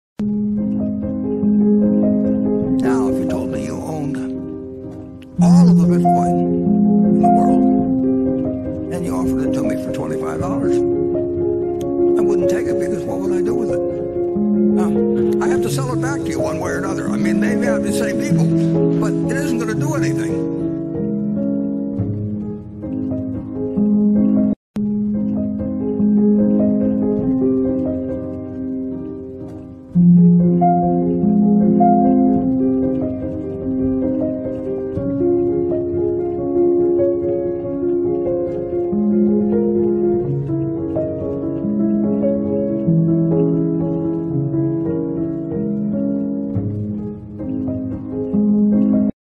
WHAT DOES WARREN BUFFET REALLY THINK ABOUT BITCOIN?
It's tough to believe Warren Buffet really truly hates Bitcoin when his company indirectly invests in it.
I still think Mr. Buffet has amazing knowledge to share with us and I really appreciate the book, "The Warren Buffet Way"
A great read!